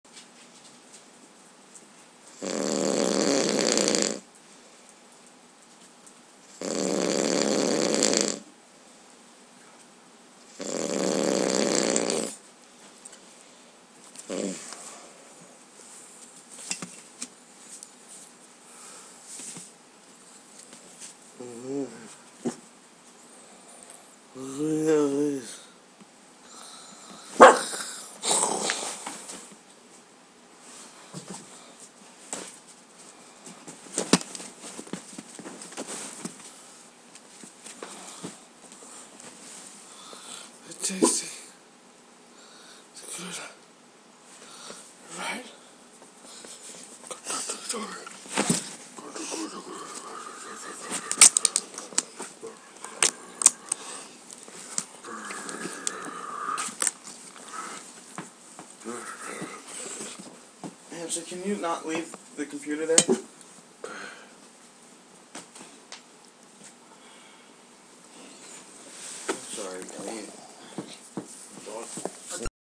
Drunk Snore DOG FACE climb
dog uses miller as a ladder. horsedick.mpeg lol